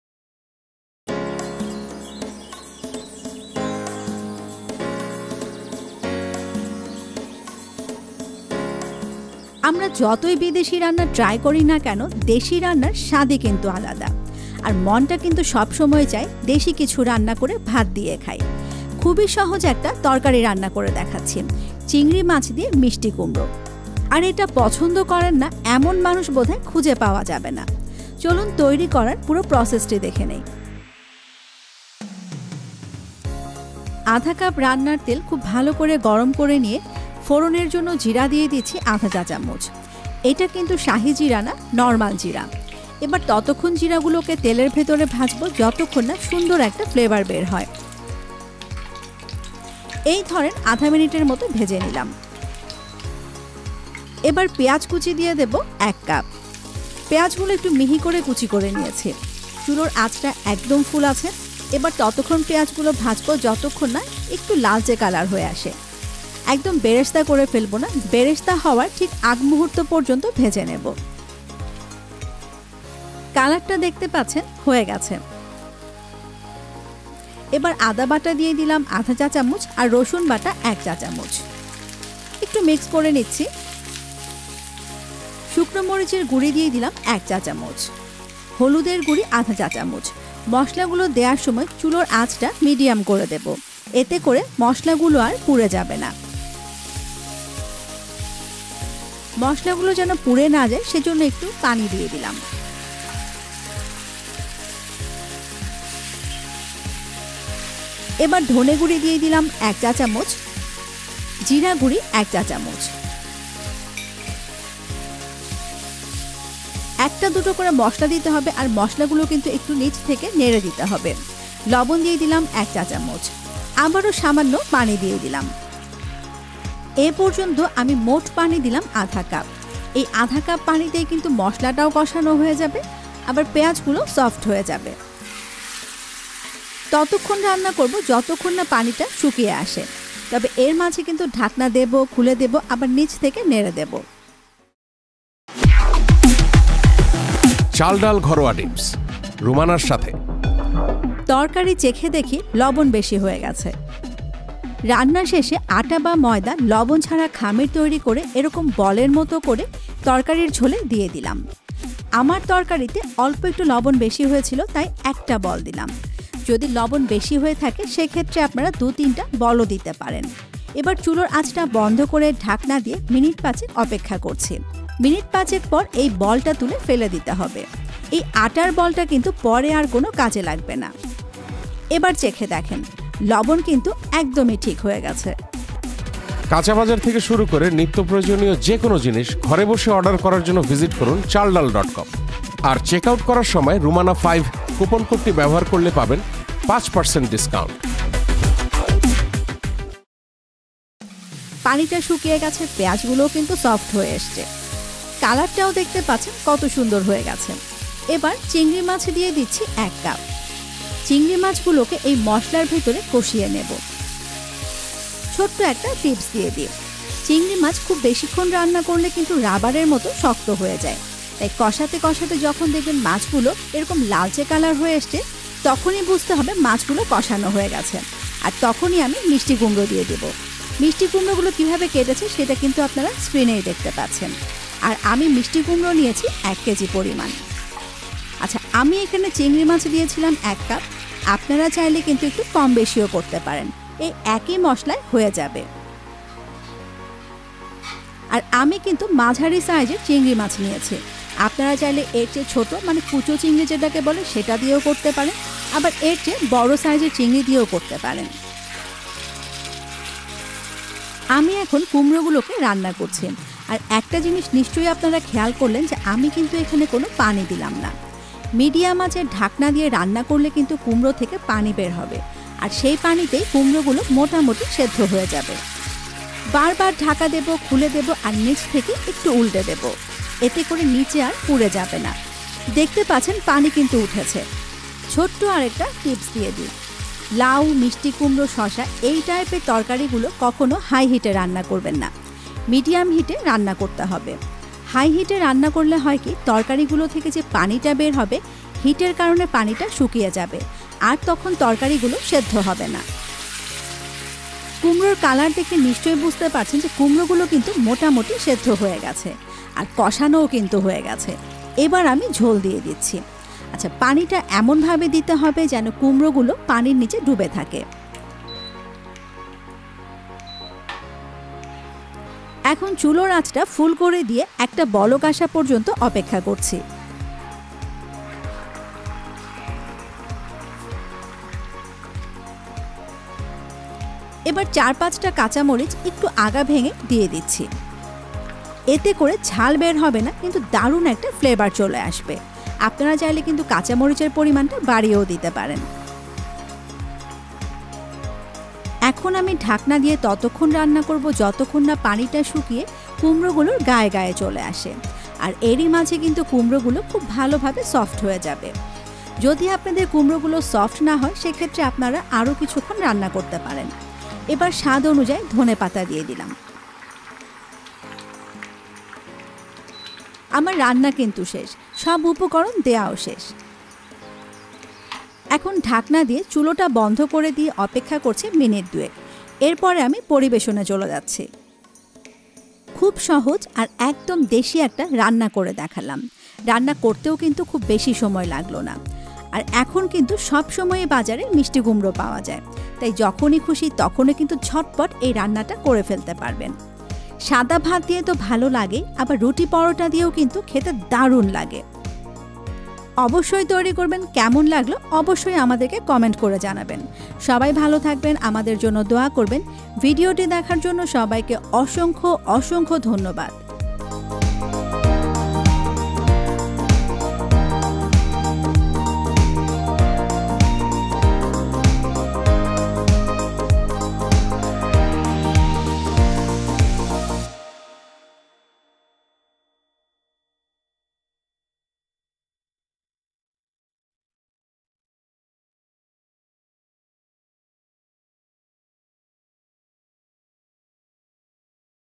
চিংড়ি মাছ দিয়ে মিষ্টি কুমড়ো তরকারি রান্না
আমাদের বাজারে মিষ্টি কুমড়ো সারা বছরই পাওয়া যায়, আর চিংড়ি মাছ দিয়ে মিষ্টি কুমড়ো আমাদের ভীষণ পছন্দের একটি রেসিপি। তৈরী করে দেখাচ্ছি চিংড়ি মাছ দিয়ে মিষ্টি কুমড়ো তরকারি।

তৈরী করতে লাগছে -
 - চিংড়ি মাছ ১ কাপ
 - মিষ্টি কুমড়ো ১ কেজি
 - রান্নার তেল ০.৫ কাপ
 - জিরা ০.৫ চা চামুচ
 - পিয়াঁজ কুচি ১ কাপ
 - আদা বাটা ০.৫ চা চামুচ
 - রসুন বাটা ১ চা চামুচ
 - শুকনো মরিচের গুঁড়ি ১ চা চামুচ
 - হলুদের গুঁড়ি ০.৫ চা চামুচ
 - ধরে গুঁড়ি ১ চা চামুচ
 - জিরা গুঁড়ি ১ চা চামুচ
 - লবণ ১ চা চামুচ
 - কাঁচা মরিচ ৪/৫ টি
 - ধনে পাতা স্বাদ অনুযায়ী

তৈরী করার অভিজ্ঞতা আমাদের ফেসবুক পেজ -এ শেয়ার করতে ভুলবেন না। আর এই রেসিপিটির ব্লগ পোস্ট আছে ঠিকানায়।